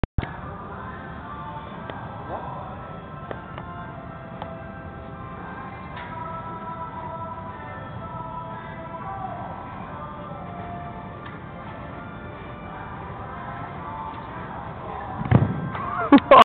LearningGuide Toekomstige docent LO
Met deze brug oefening is helemaal NIX mis mee. Vooral die afsprong is van grote klasse. Iedere docent LO zou dit willen kunnen ;)